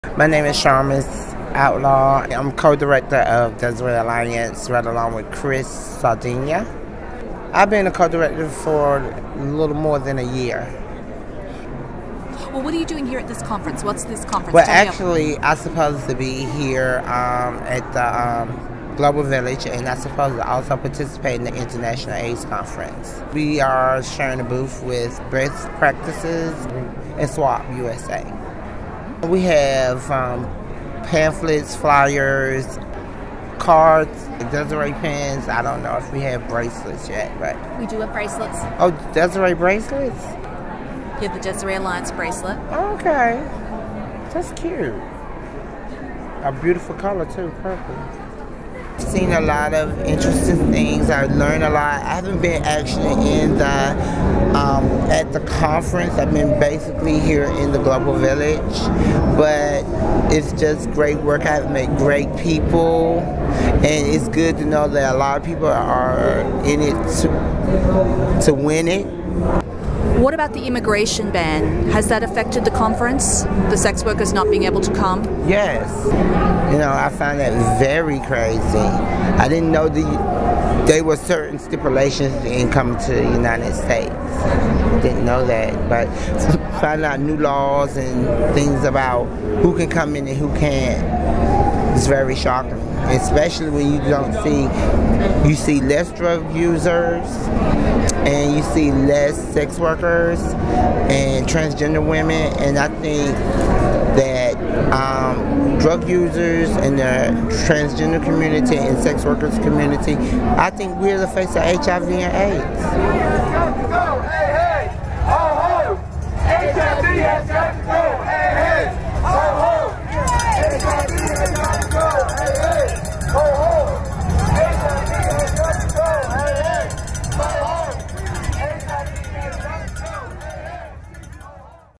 Sharmus Outlaw (Desiree Alliance) during the International AIDS Conference in 2012
In 2012 US representatives of sex worker community, trans communities and drug users attended the International AIDS Conference in Washington, DC. Many international representatives were unable attend however due to US entrance restrictions and held conferences and festival in other countries. Sharmus Outlaw of the Desiree Alliance provides her thoughts about the conference and the immigration ban.